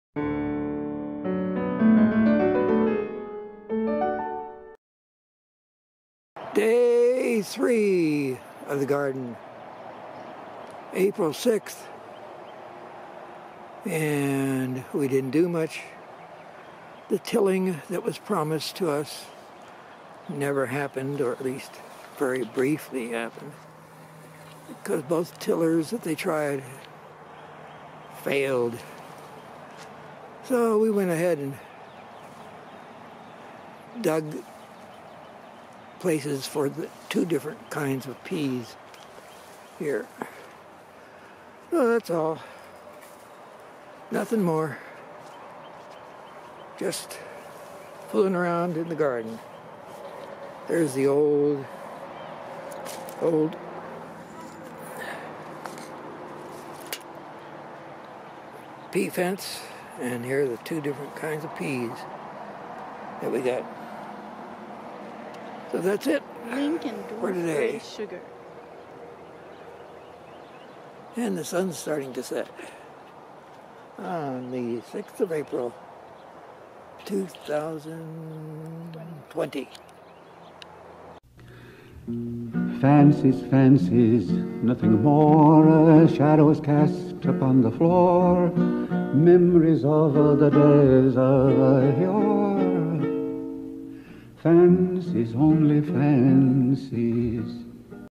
03 Garden Peas 2020 April 6
Our third day in the Azalea Garden plot - we wanted to have it tilled, but two borrowed tillers broke down. So we forked up a central row and planted peas of two varieties on each side of the pea fence. Also looked at the tadpoles and other garden plots, watch others taking away our scarce mulch, enjoyed watching the almost-full moon rising above our Azalea Park.

Origininal file = 03GardenPeas1m38s.mp4 Length = 1m 38s. Philip Mc pmhilipmc